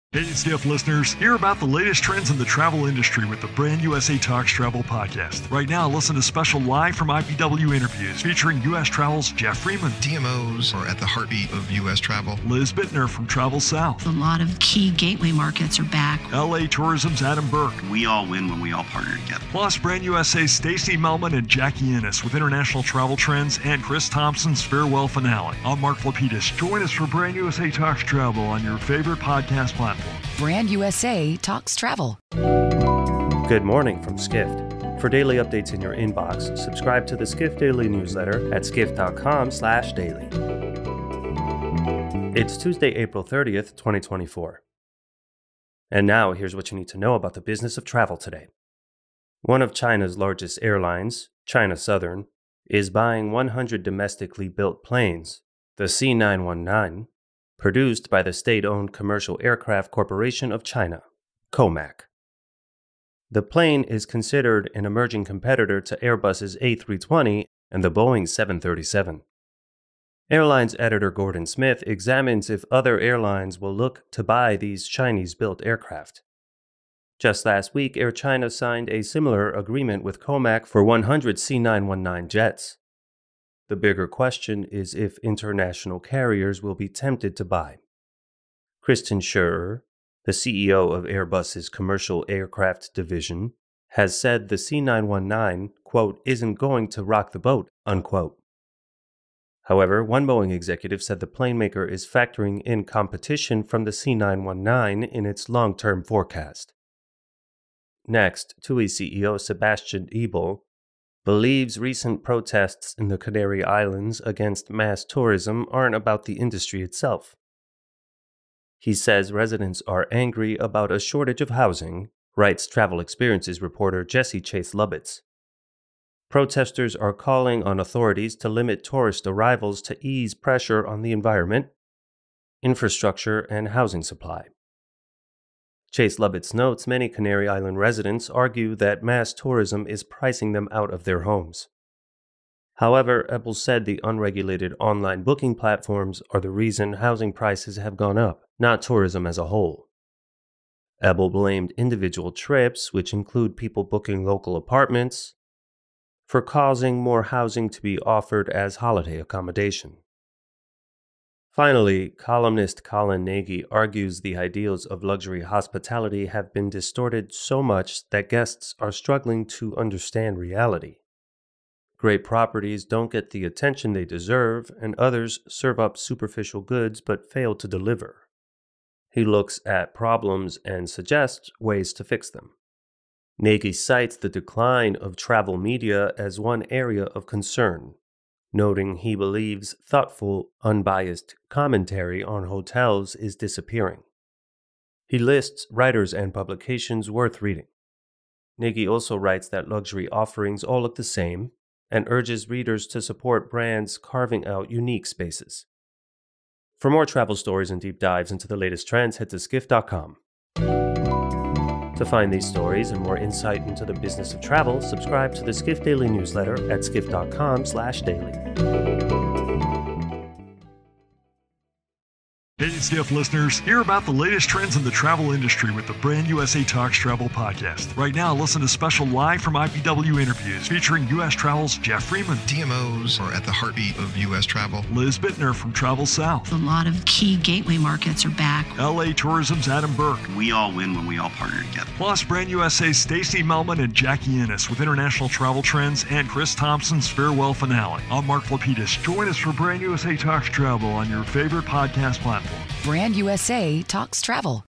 China’s New Plane Wins Could Signal More Competition for Airbus and Boeing
Episode Notes

One of China’s largest airlines, China Southern, is buying 100 domestically-built planes – the C919, produced by the state-owned Commercial Aircraft Corporation of China (COMAC).

The plane is considered an emerging competitor to Airbus’ A320 and the Boeing 737. And Airlines Editor Gordon Smith examines if other airlines will look to buy these Chinese-built aircraft.

Just last week, Air China signed a similar agreement with COMAC for 100 C919 jets. The bigger question is if international carriers will be tempted to buy.

Christian Scherer, the CEO of Airbus’ commercial aircraft division, has said the C919 “isn’t going to rock the boat.” However, one Boeing executive said the planemaker is factoring in competition from the C919 in its long-term forecast.

Next, TUI CEO Sebastian Ebel believes recent protests in the Canary Islands against mass tourism aren’t about the industry itself He says residents are angry about a shortage of housing, writes Travel Experiences Reporter Jesse Chase-Lubitz.

Protestors are calling on authorities to limit tourist arrivals to ease pressure on the environment, infrastructure and housing supply. Chase-Lubitz notes many Canary Islands residents argue that mass tourism is pricing them out of their homes.

However, Ebel said the unregulated online booking platforms are the reason housing prices have gone up — not tourism as a whole. Ebel blamed individual trips, which include people booking local apartments, for causing more housing to be offered as holiday accommodation.

Finally, columnist Colin Nagy argues the ideals of luxury hospitality have been distorted so much that guests are struggling to understand reality : Great properties don’t get the attention they deserve, and others serve up superficial goods but fail to deliver. He looks at the problems and suggests ways to fix them.

Nagy cites the decline of travel media as one area of concern, noting he believes thoughtful, unbiased commentary on hotels is disappearing. He lists writers and publications worth reading. Nagy also writes that luxury offerings all look the same, and urges readers to support brands carving out unique spaces.

Producer/Presenter: Jose Marmolejos